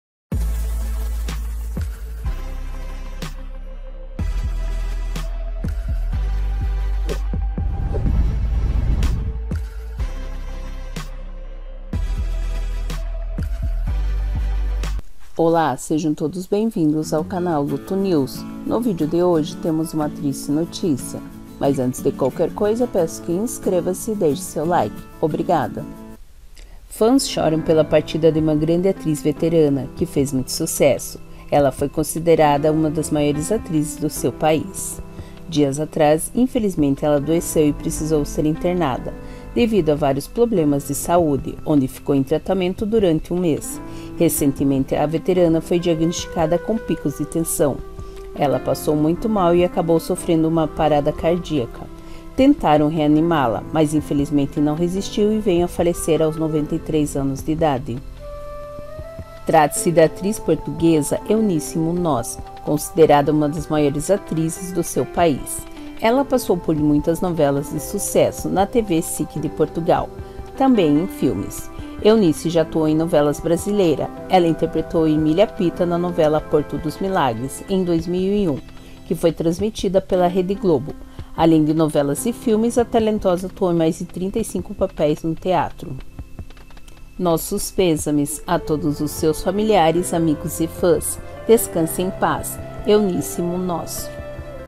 TV EM LUTO MORREU AGORA POUCO ATRIZ QUE FEZ VÁRIAS NOVELAS DE SUCESSO "ERA UMA DAS MAIORES"
OLÁ SEJAM TODOS BEM VINDOS AO CANAL LUTO NEWS NO VIDEO DE HOJE TEMOS UMA TRISTE NOTÍCIA, MAIS ANTES DE QUALQUER COISA PEÇO QUE INSCREVA-SE E DEIXE O SEU LIKE OBRIGADA.

FÃS CHORAM PELA PARTIDA DE UMA GRANDE ATRIZ VETERANA QUE FEZ MUITO SUCESSO, ELA FOI CONSIDERADA UMA DAS MAIORES ATRIZES DO SEU PAÍS.

DIAS ATRÁS INFELIZMENTE ELA ADOECEU E PRECISOU SER INTERNADA DEVIDO A VÁRIOS PROBLEMAS DE SAÚDE ONDE FICOU EM TRAMENTO DURANTE UM MÊS, RECENTECENTE A VETERANA FOI DIAGNÓSTICADA COM PICOS DE TENSÃO, ELA PASSOU MUITO MAL E ACABOU SOFRENDO UMA PARADA CARDÍACA, TENTARAM REANIMÁ-LA MAIS INFELIZMENTE NÃO RESISTIU E  VENHO A FALECER AOS 93 ANOS DE IDADE.

TRATA SE SA ATRIZ PORTUGUESA EUNICE MUÑOZ CONSIDERADA UMA DAS MAIORES ATRIZES DO PAÍS ELA PASSOU POR MUITAS NOVELAS DE SUCESSO NA TV SIC DE PORTUGAL, TAMBÉM FILMES, EUNICE JÁ ATUOU EM NOVELAS BRASILEIRAS, ELA INTERPRETOU EMILIA PITA NA NOVELA PORTO DOS MILAGRES EM 2001 QUE FOI TRANSMITIDA PELA REDE GLOBO, ALÉM DE NOVELAS E FILMES A TALENTOSA ATUOU EM MAIS DE 35 PAPÉIS NO TEATRO.

NOSSOS PÊSAMES A TODOS SEUS FAMILIARES, AMIGOS E FÃS DESCANSE EM PAZ EUNICE MUÑOZ.